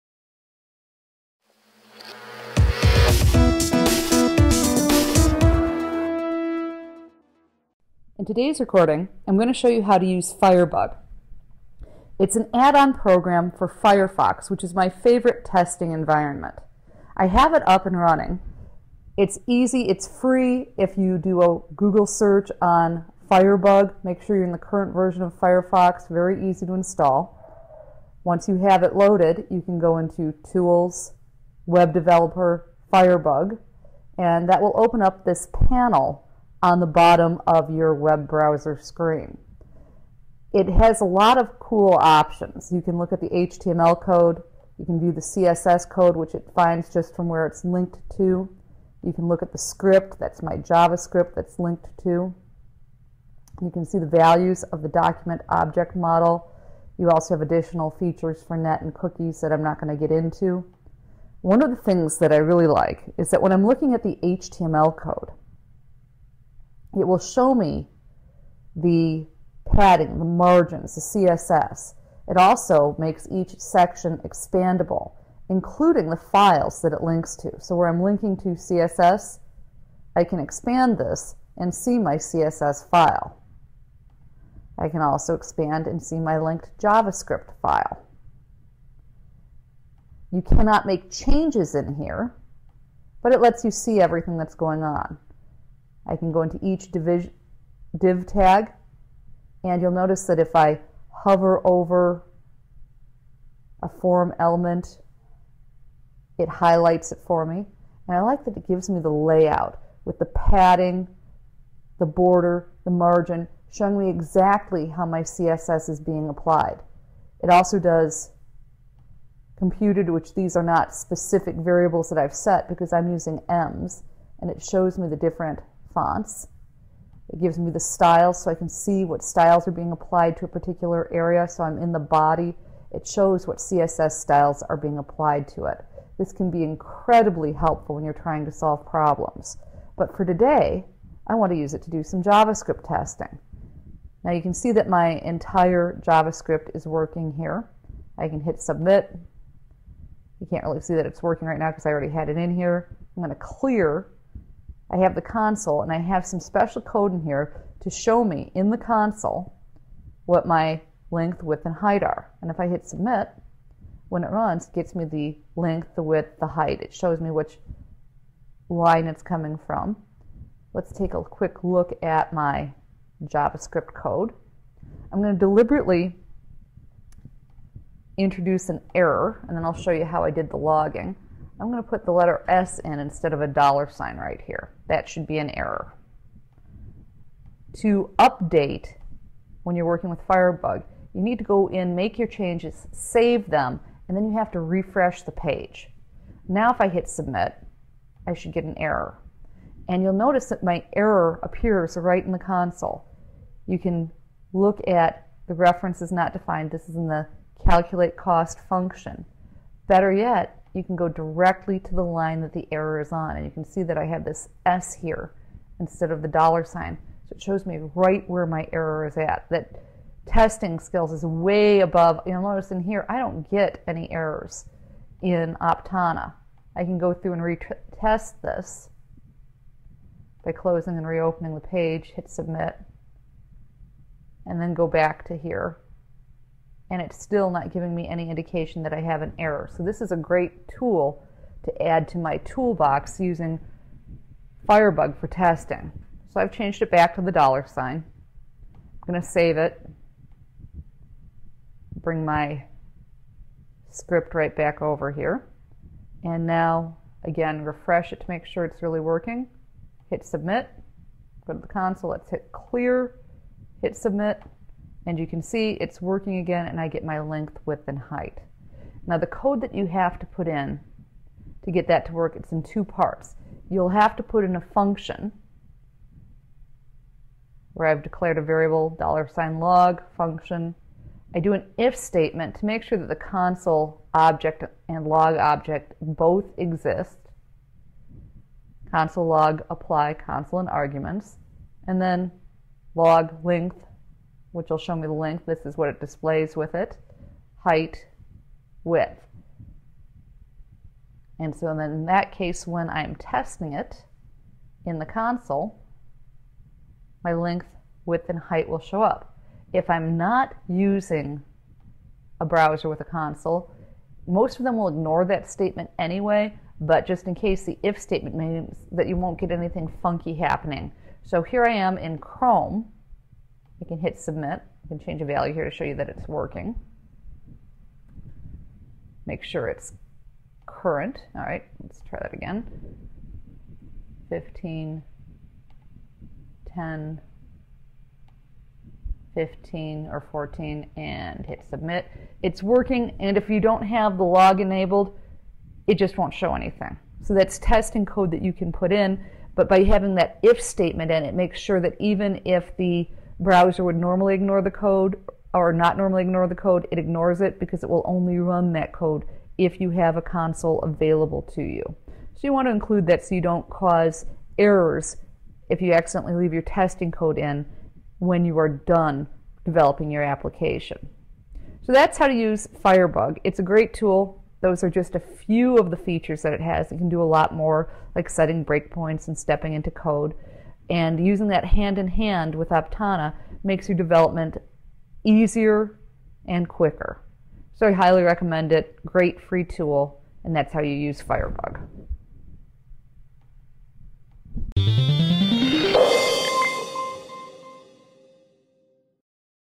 merihelp.net: Debugging JavaScript with firebug
merihelp.net: JavaScript - Using firebug to help you debug your JavaScript code